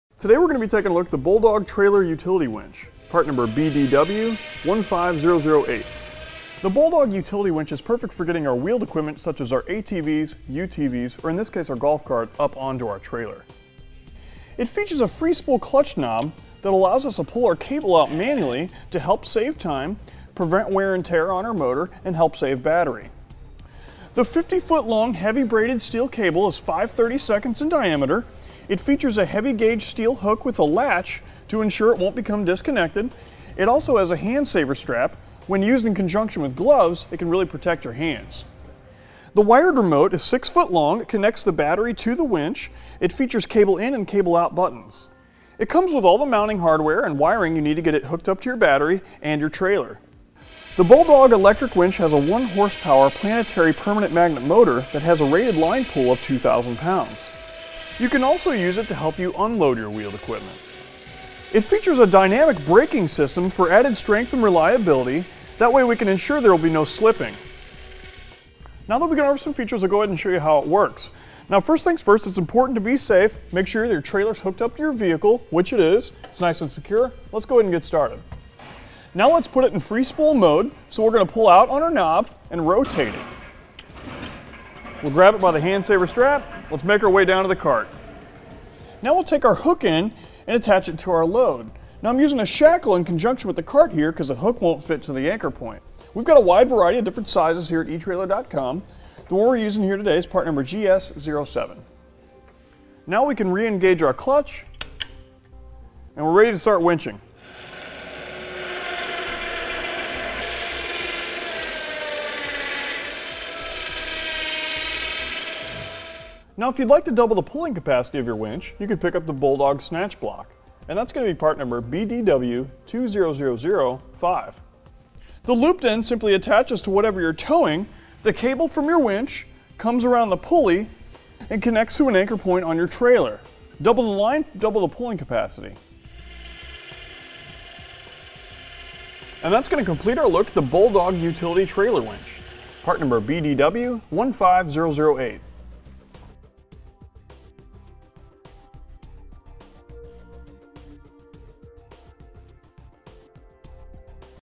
etrailer | Bulldog Trailer Winch with Hand Held Controller Review
Today we're going to be taking a look at the Bulldog trailer utility winch, part BDW15008. The Bulldog utility winch is perfect for getting our wheeled equipment such as our ATVs, UTVs, or in this case, our golf cart up onto our trailer. It features a free spool clutch knob that allows us to pull our cable out manually to help save time, prevent wear and tear on our motor and help save battery. The 50 ft. long braided steel cable is 5/32nds in diameter. It features a heavy gauge steel hook with a latch to ensure it won't become disconnected.

It also has a hand saver strap when used in conjunction with gloves, it can really protect your hands. The wired remote is 6 ft. long. It connects the battery to the winch. It features cable in and cable out buttons.

It comes with all the mounting hardware and wiring you need to get it hooked up to your battery and your trailer. The Bulldog electric winch has a 1 horsepower planetary permanent magnet motor that has a rated line pull of 2,000 lbs. You can also use it to help you unload your wheeled equipment. It features a dynamic braking system for added strength and reliability. That way we can ensure there'll be no slipping. Now that we've gone over some features, I'll go ahead and show you how it works.

Now first things first, it's important to be safe. Make sure that your trailer is hooked up to your vehicle which it is. It's nice and secure. Let's go ahead and get started. Now let's put it in free spool mode so we're going to pull out on our knob and rotate it.

We'll grab it by the hand saver strap, let's make our way down to the cart. Now we'll take our hook end and attach it to our load. Now I'm using a shackle in conjunction with the cart here because the hook won't fit to the anchor point. We've got a wide variety of different sizes here at etrailer.com. The one we're using here today is part GS07. Now we can re-engage our clutch and we're ready to start winching. Now if you'd like to double the pulling capacity of your winch, you can pick up the Bulldog snatch block and that's going to be part BDW20005. The looped end simply attaches to whatever you're towing. The cable from your winch comes around the pulley and connects to an anchor point on your trailer. Double the line, double the pulling capacity. That's going to complete our look at the Bulldog utility trailer winch, part BDW15008.